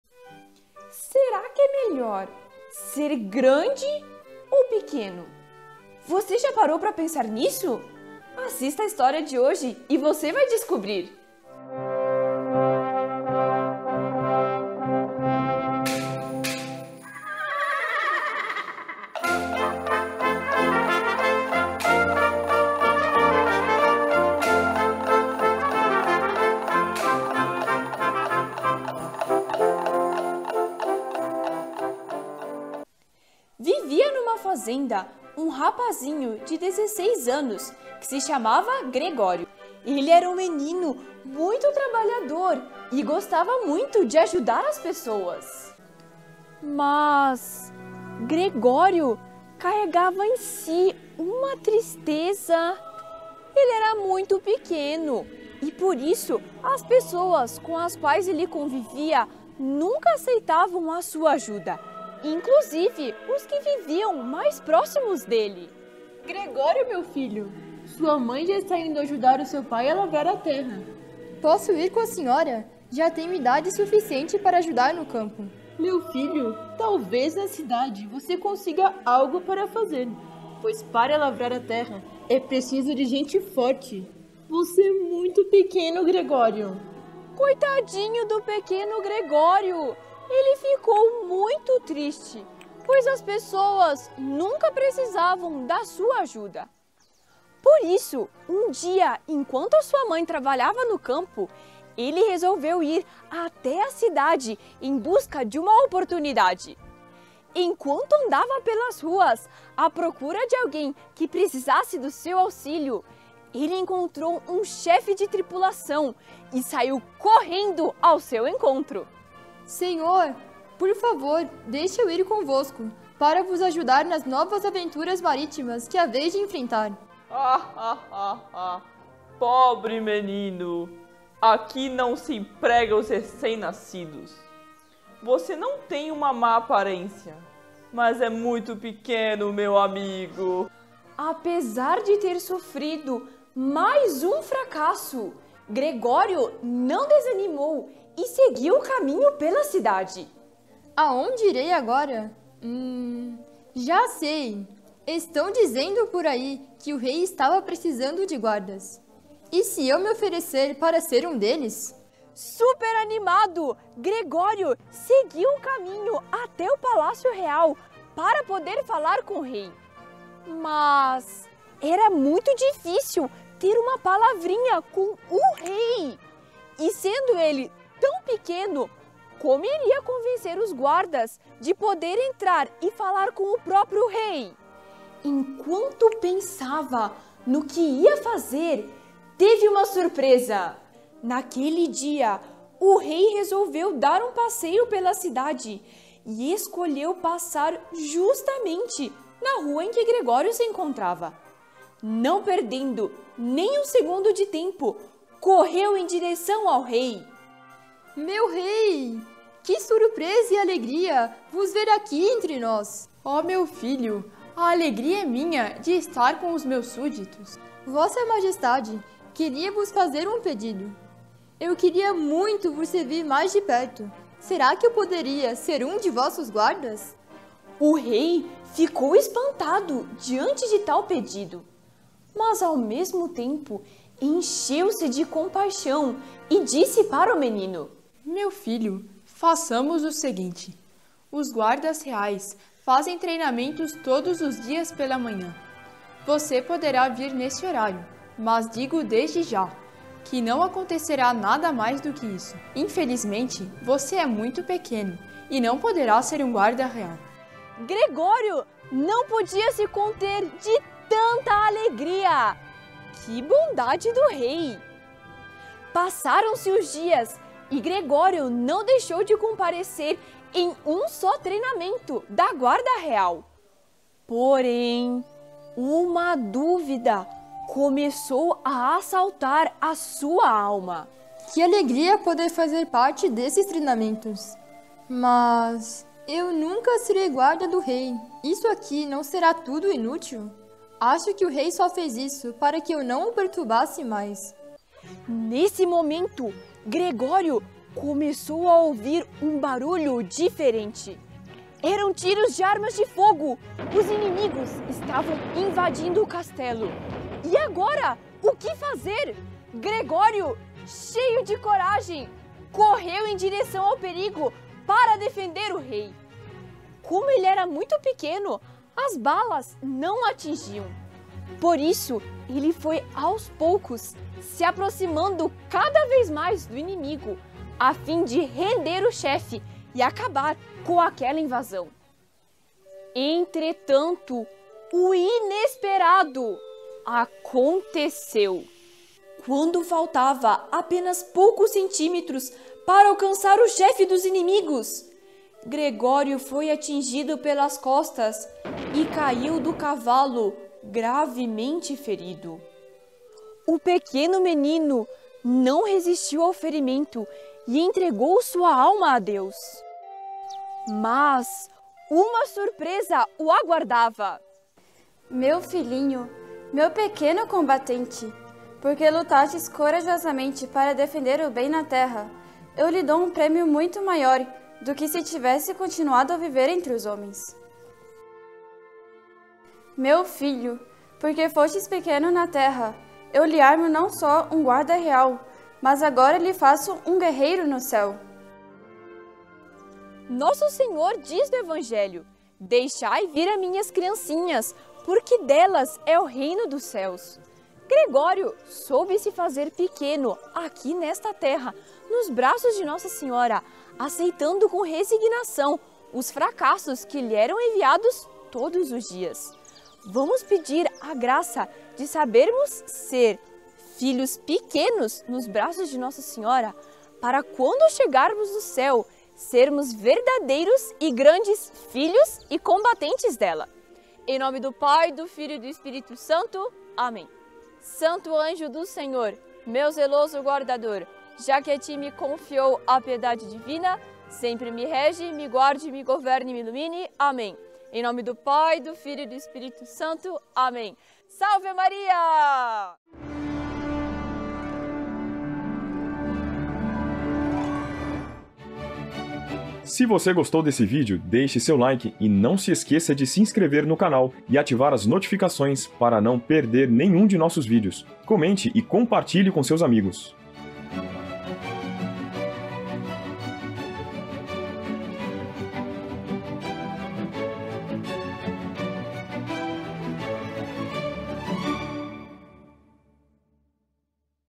O pequeno Gregório ⎜História para crianças
Gregório era muito pequeno e ninguém aceitava a sua ajuda. Certa manhã, encontrou-se com o rei que passeava pela rua e Gregório se ofereceu para ser um dos guardas reais. O rei cheio de compaixão, disse que ele poderia apenas participar dos treinos. Um dia, os inimigos invadiram o castelo, e Gregório foi gravemente ferido, entregando sua alma a Deus. No Céu, teve uma surpresa.